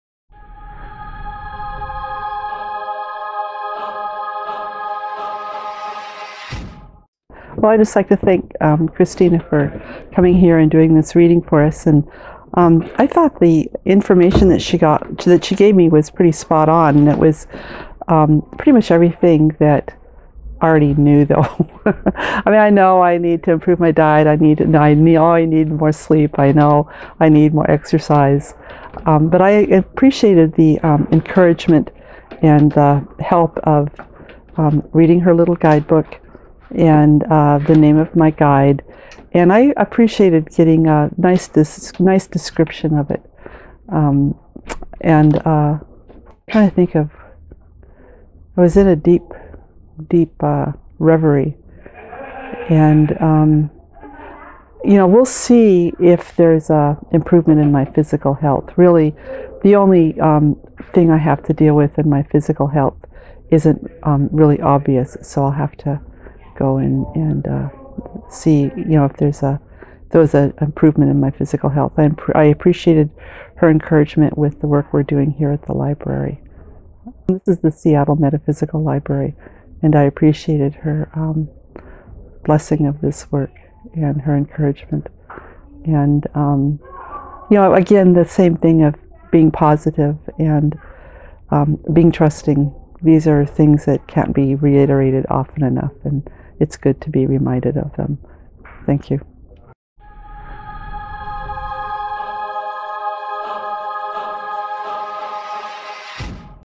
Testimonial - Margaret Bartley (President of The Seattle Metaphysical Library)
Athella is an Ascended Master, Angel, Spirit, or Alien. She says that there is no difference between these concepts, these labels are just for human minds, although she prefers to be seen as an Angel.

Athella shares with us that there are many dimensions, and that the universe is like a tall building, where every floor is a dimension and every dimension is a different reality. Humans are on the 3rd floor and Athella is on the 12th floor. Athella talks to people through Christina's body using a technique known as channeling.